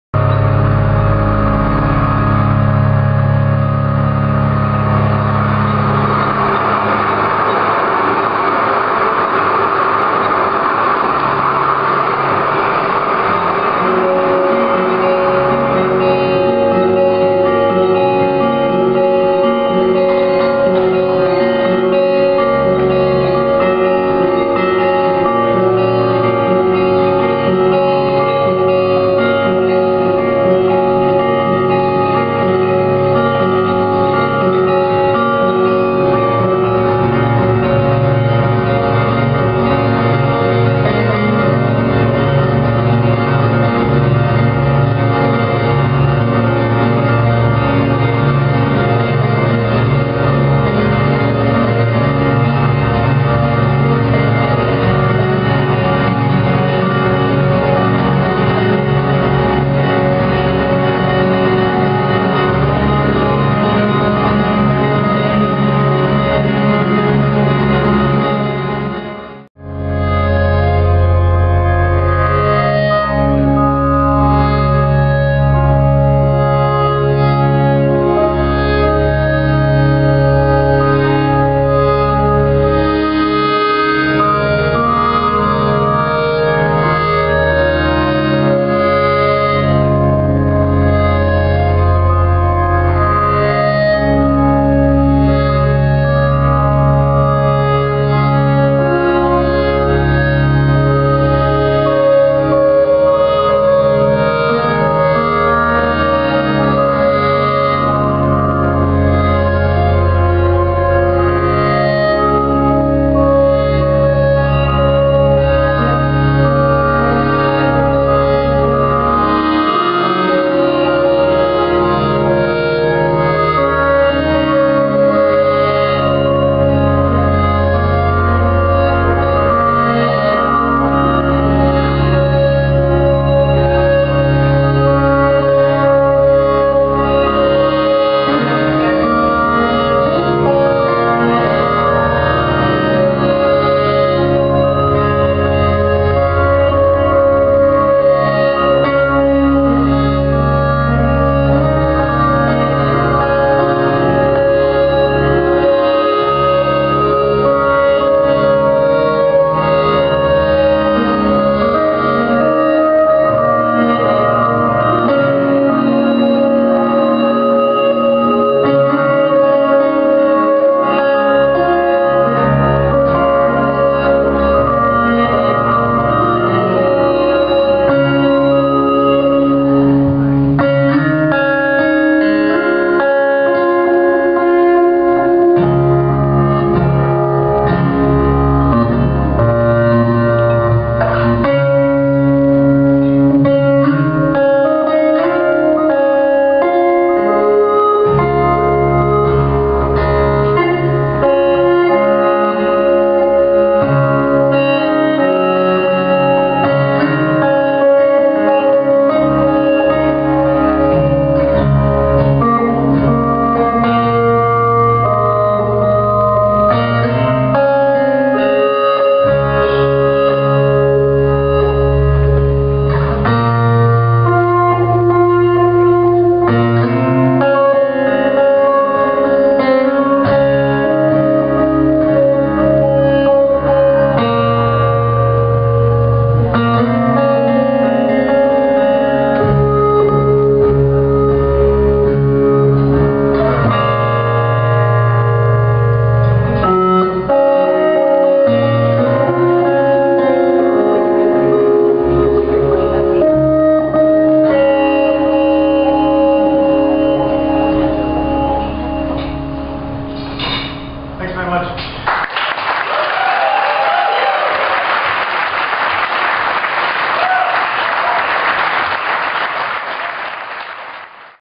Mountains in Seattle 9/27/09: Decibel Fest
Here are Mountains performing at the Triple Door in Seattle for Decibel Fest.